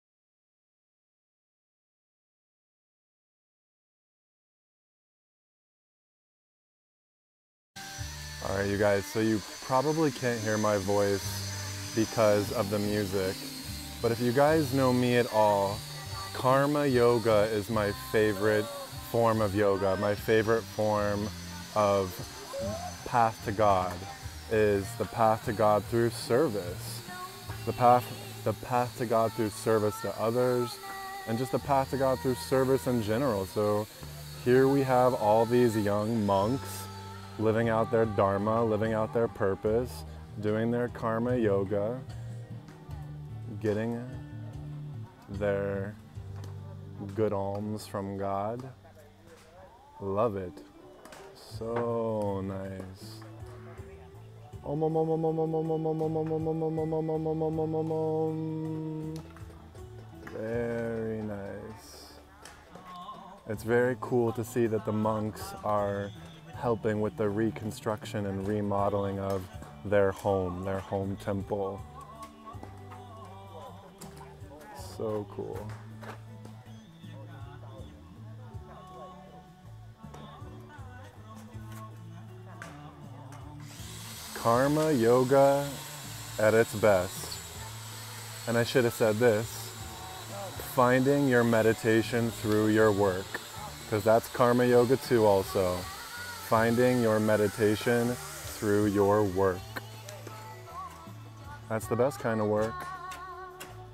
Great Karma Yoga Example with the Wat Bo Buddhist Monks; Yogi Parks Spiritual Pilgrimage, Cambodia
I absolutely loved this moment, of the Wat Bo Temple, Buddhist Monks, giving such a great example of Karma Yoga. Karma Yoga-The Path to God or "Self" Realization through "Divine Service" for others.

Omazing!- Yogi Parks